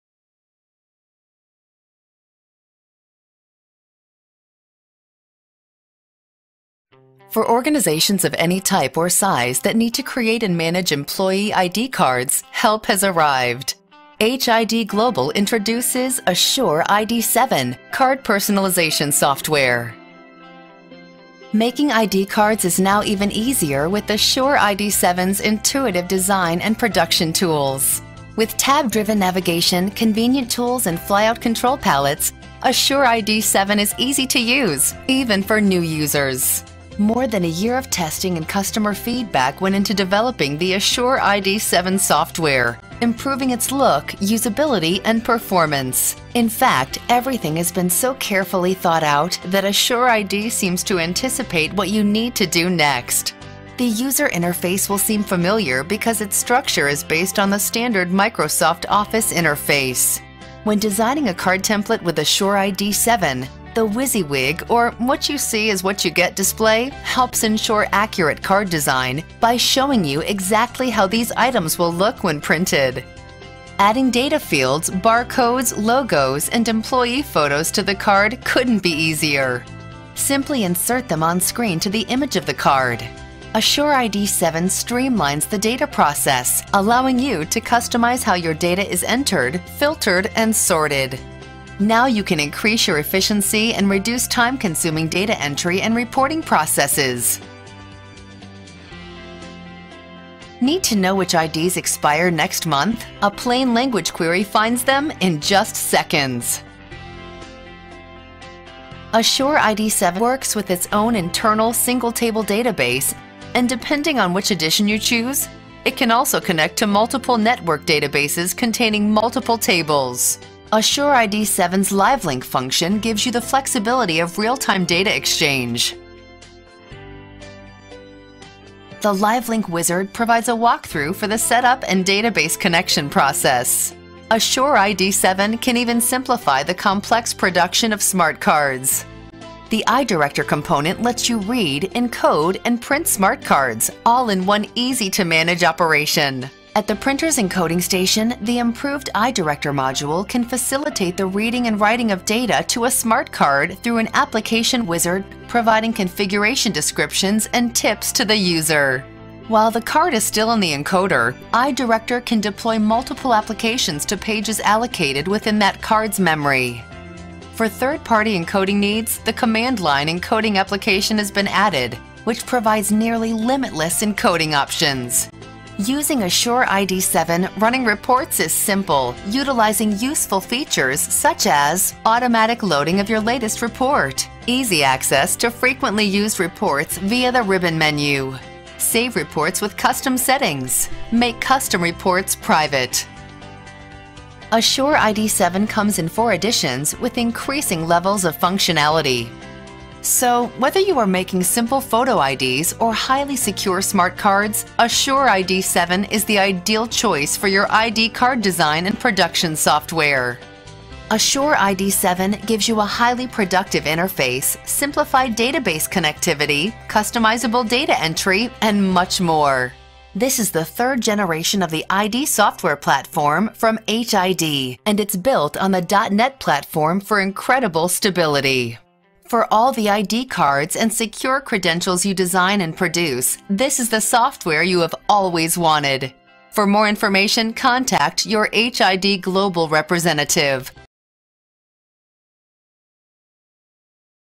Asure ID Card Personalization Software for printing with HID Badge Maker Machines
The newest generation of Asure ID photo ID software is here with Asure ID 7!

Featuring a friendly and intuitive interface that makes photo ID card design and data entry a breeze. Choose Your Source and easily print plastic ID cards from records stored in the internal database. With real time data exchange through Live Link, you can share across multiple workstations directly from your network’s HR, security or ERP database, adding Oracle, MySQL, LDAP and DVTel support.

The iDIRECTOR® encoding module improves smart card development and deployment and supports iCLASS® and HID PROX, as well as MIFARE® and DESFire® card serial number (CSN) reading and you can multi-task with tabbed navigation, while effortlessly switching between ID card design and data entry, managing each independently.

Asure ID 7 provides a dramatic speed boost, and everything you need to design and populate your ID cards is right in front of you in a crisp, clean environment that allows you to glide from data entry to ID card design to printing and reporting! Asure ID 7 fits seamlessly into your existing ID card system!